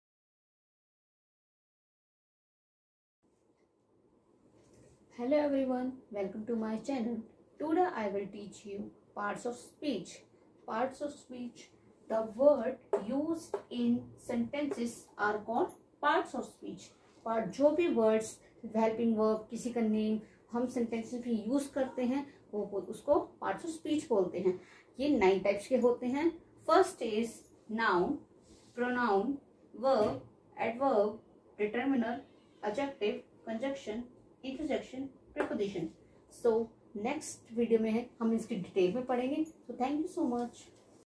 Parts of Speech | Learn with Sunaina Mam
The words used in sentences are called Parts of Speech

1) Noun
2) Pronoun
3) Verb
4) Adverb
5) Determiner
6) Adjective
7) Conjunction
8) Interjection
9) Preposition